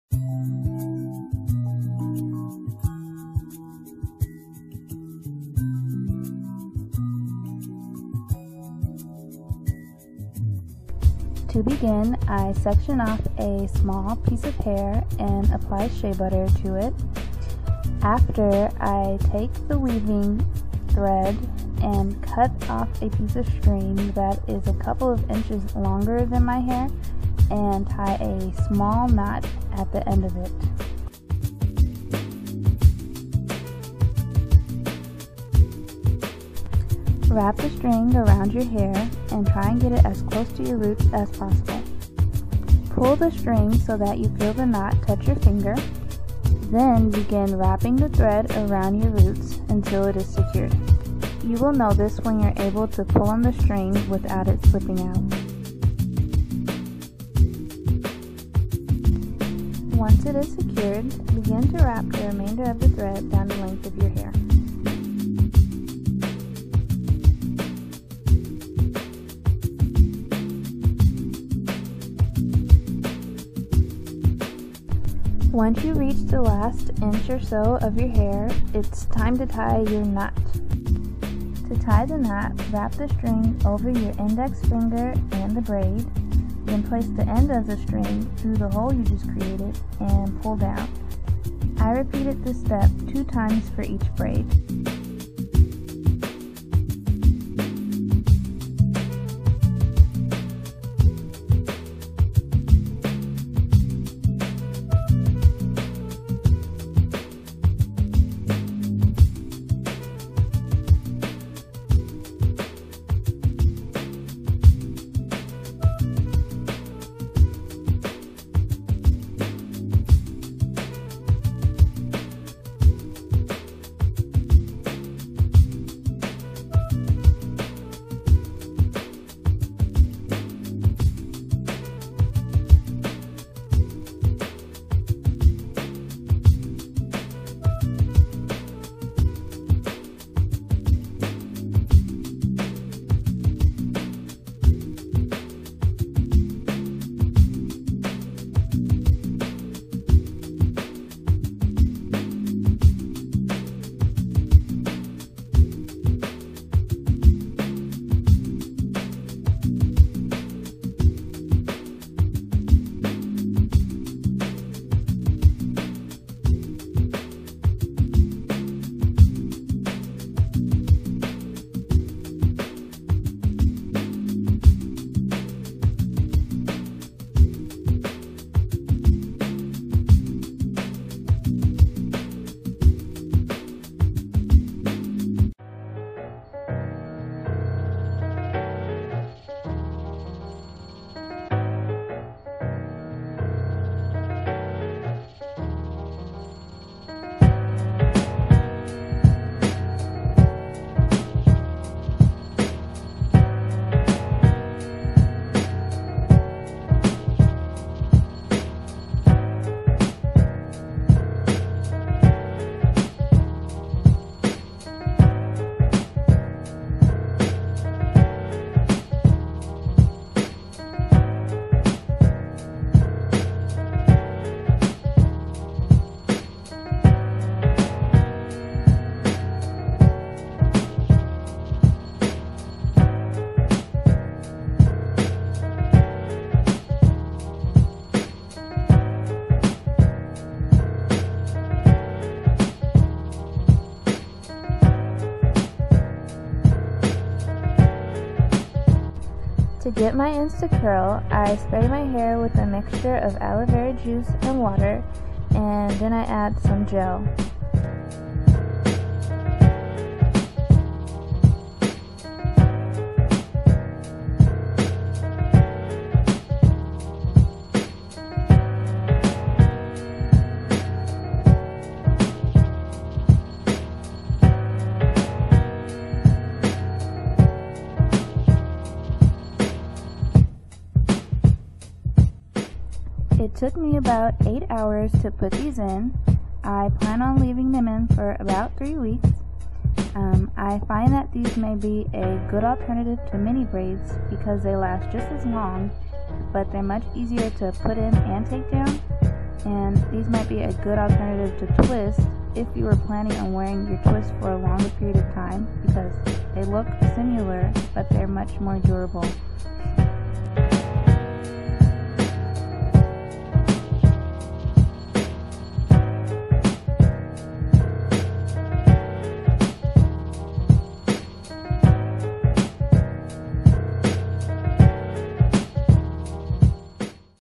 *42*African Threading on Natural Hair (Alternative to Individual Braids)
Hey everyone! In this video I will be showing you how I African threaded my hair. The whole process took me about 8 hours to complete and I plan on leaving them in for about three weeks. All you will